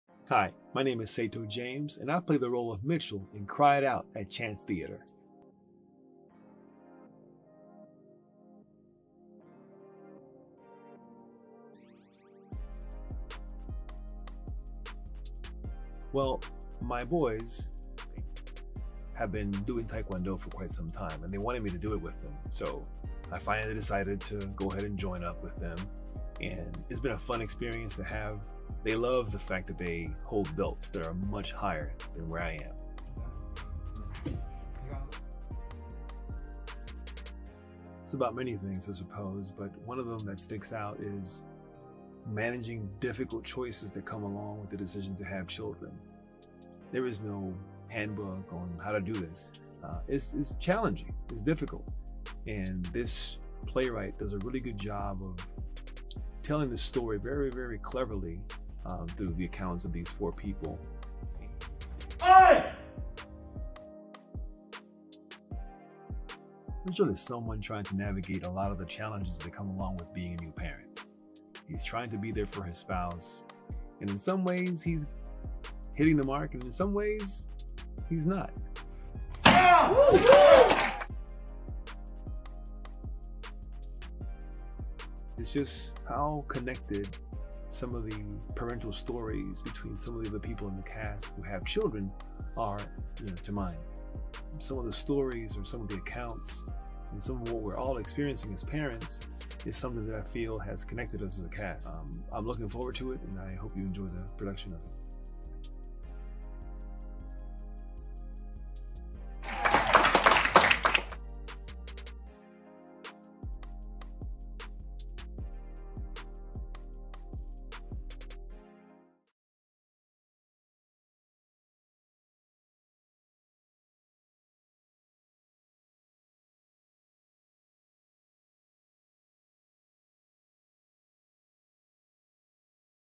"Cry It Out" Artist Interviews: Seyto James as Mitchell
Meet Seyto James, who plays Mitchell in our OC premiere of "Cry It Out," a comedy by Molly Smith Metzler that is for parents and those of us who love them!

Seyto talks about how he and the cast are bonding over their parental experiences and how he and his sons have taken up a similar hobby...

TIX/INFO ► Chancetheater.com/wah

ABOUT "CRY IT OUT"...
Acclaimed writer Molly Smith Metzler (“Shameless,” “Orange is the New Black,” and “Maid”) holds both a microscope and a megaphone to the joys and perils that today’s mothers face. This hilarious and heart-warming comedy takes a sharp and honest look at the power of female friendship, the dilemma of going back to work after being home with a newborn, and the effect that economic and social class has on parenthood in America.

NOTE: This show contains adult themes and language.